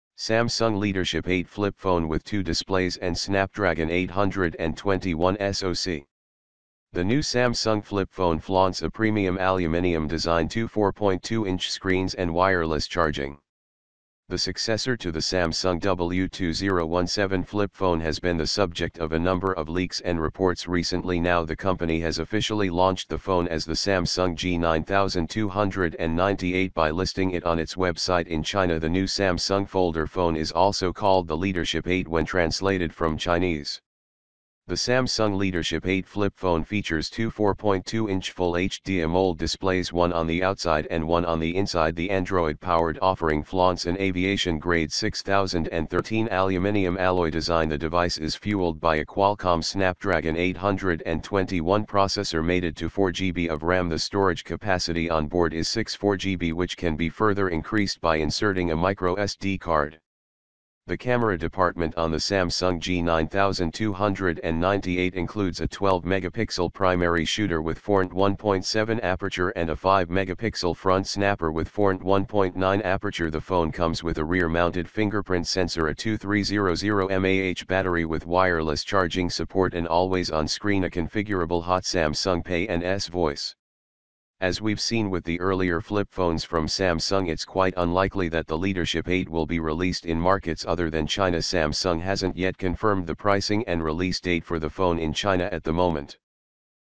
Samsung leadership 8 turn cellphone with two presentations
SAUS Ultra-Slim QI Wireless Charger
Universal Waterproof Case
Universal Waterproof Case Firefish

Samsung Leadership 8 flip phone with two displays and Snapdragon 821 SoC
The new Samsung flip phone flaunts a premium aluminium design two 4.2 inch screens and wireless charging
The successor to the Samsung W2017 flip phone has been the subject of a number of leaks and reports recently Now the company has officially launched the phone as the Samsung G 9298 by listing it on its website in China The new Samsung folder phone is also called the Leadership 8 when translated from Chinese
The Samsung Leadership 8 flip phone features two 4.2 inch full HD AMOLED displays one on the outside and one on the inside The Android powered offering flaunts an aviation grade 6013 aluminium alloy design The device is fuelled by a Qualcomm Snapdragon 821 processor mated to 4GB of RAM The storage capacity on board is 64GB which can be further increased by inserting a microSD card
The camera department on the Samsung G 9298 includes a 12 megapixel primary shooter with fornt 1.7 aperture and a 5 megapixel front snapper with fornt 1.9 aperture The phone comes with a rear mounted fingerprint sensor a 2300mAh battery with wireless charging support an always on screen a configurable hotkey Samsung Pay and S Voice
As we’ve seen with the earlier flip phones from Samsung it’s quite unlikely that the Leadership 8 will be released in markets other than China Samsung hasn’t yet confirmed the pricing and release date for the phone in China at the moment